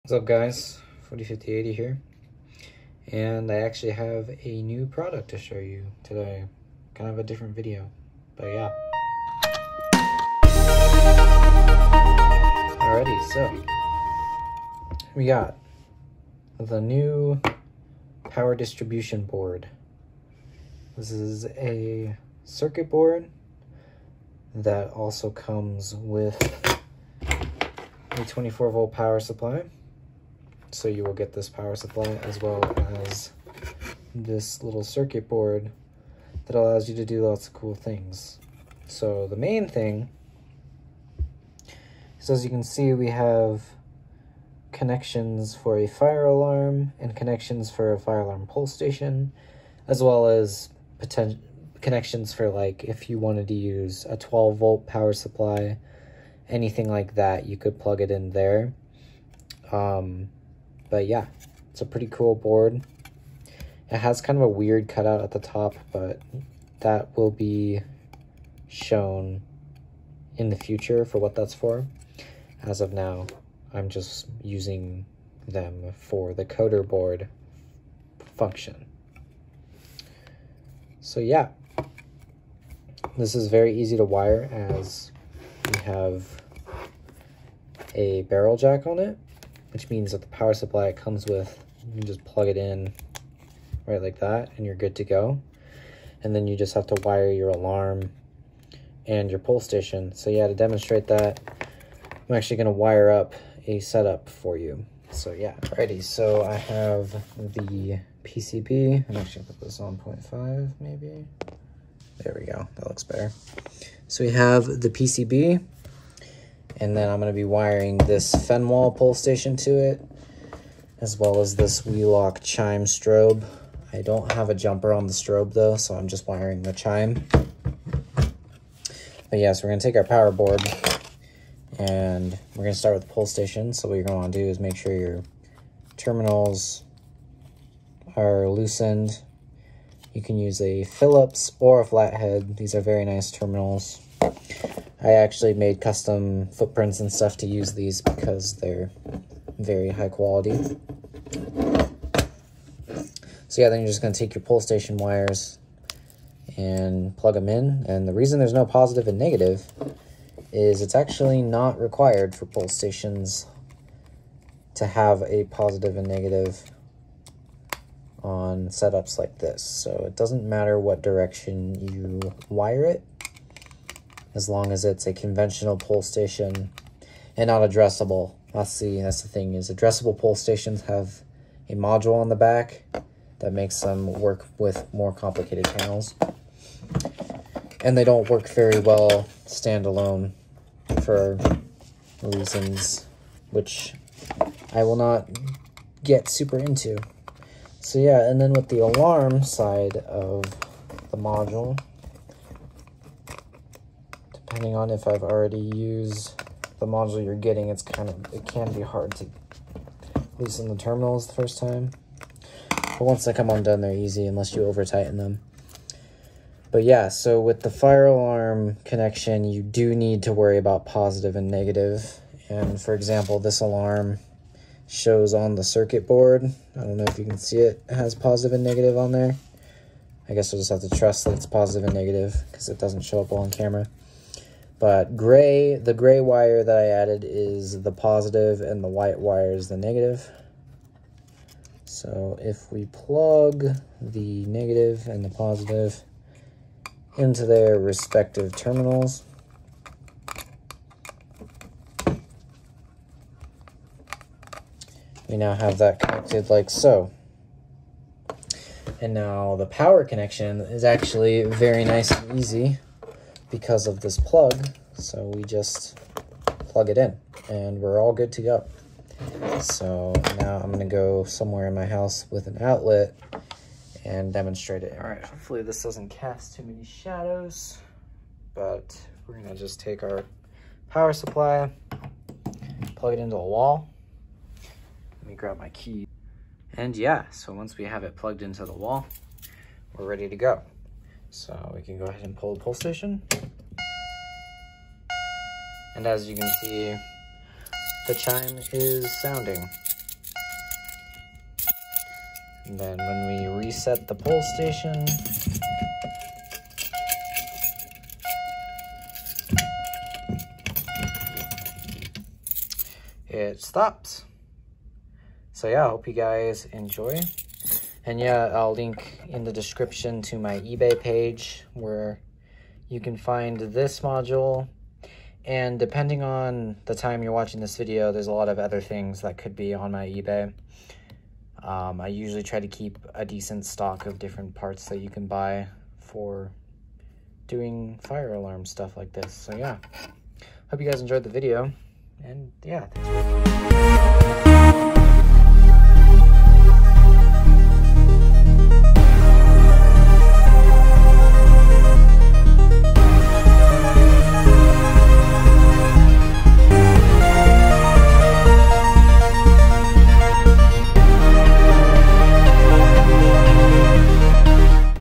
New Product! PDB-1224 Power Distribution Board
You ask and you shall receive! If you saw the poll I made around 9 days ago this is the device I was advertising. I hope it can make collecting and running devices a lot easier. Dusmc connectivity coming soonish

Check out my ebay store to see if this module is currently in stock

chapters:
00:00 Intro
00:12 Overview
01:45 Wiring Demonstration